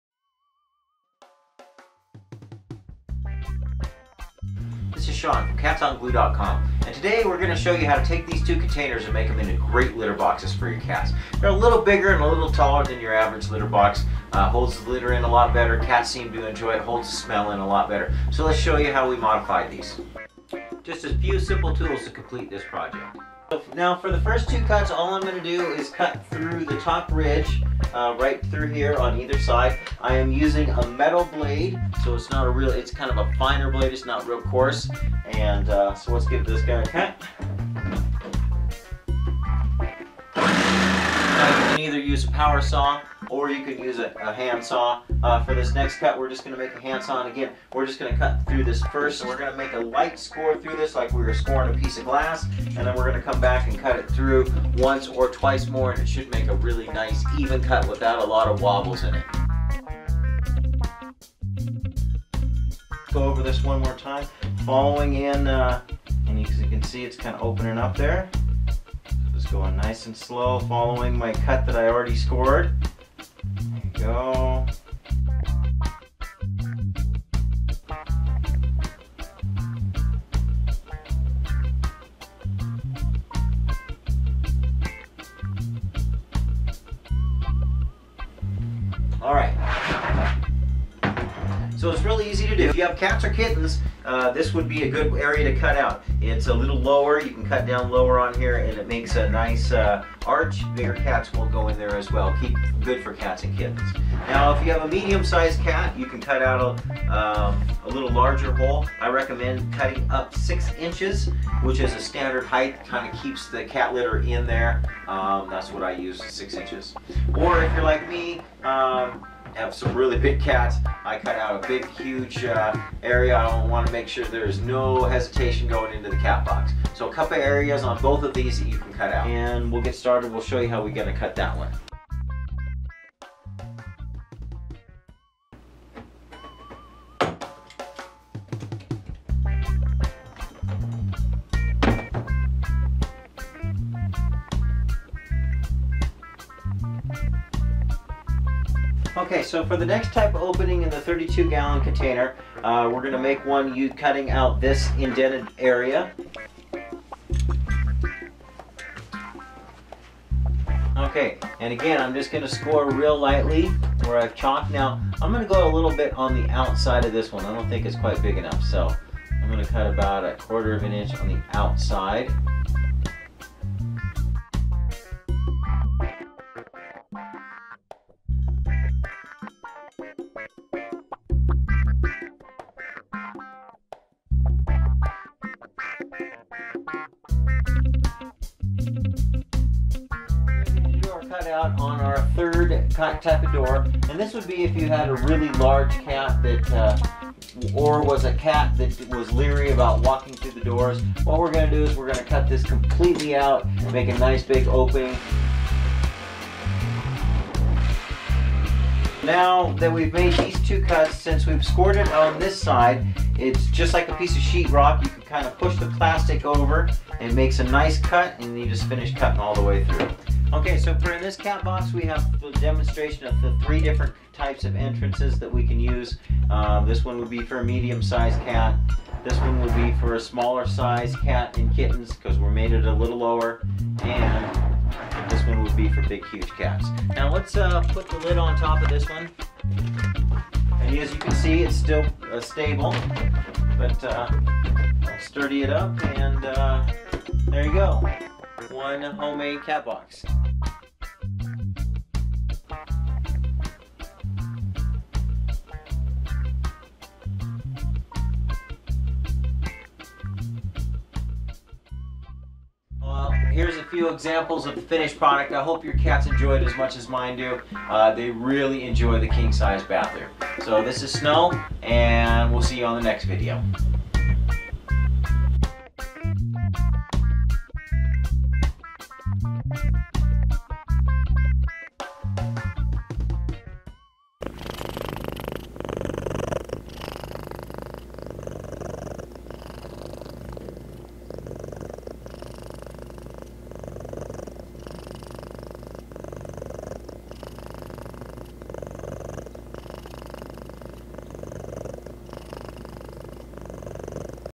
How To Make A King Size Cat Box
Make your cats a king sized cat box by using 32 and 64 gallon containers. This is great for large breed cats.  If you have cats that peeee over the edge of the cat box this will help.  You can even stack these boxes. This box is perfect for that cat who throws its litter everywhere.